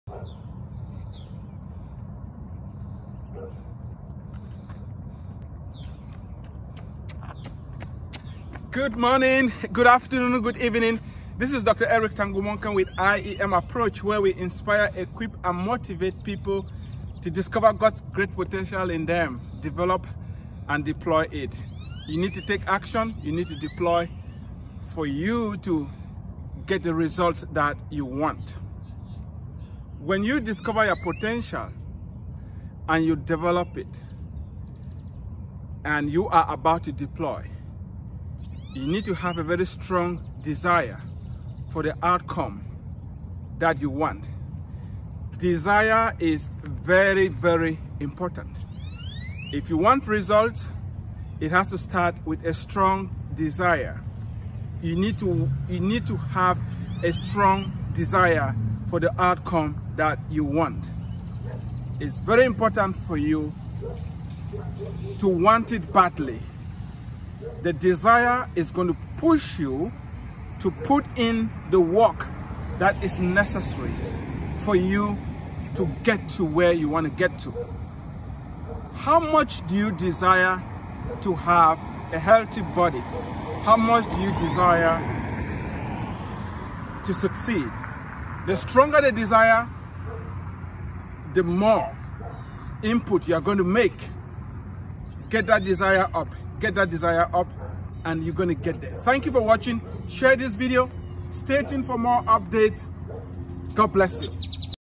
You need a strong desire
You need to have a strong desire for the outcome that you want. Without a strong and compelling desire you will not put in the hard work that is needed for you to obtain the results that you want.

Here is snorting that you may want to consider as you develop the desire for whatever you want.

Take delight in the Lord,    and he will give you the desires of your heart. Psalm 37:4

Never forget that you are not alone because God has promised to grant you the desires of your heart if you delight in Him.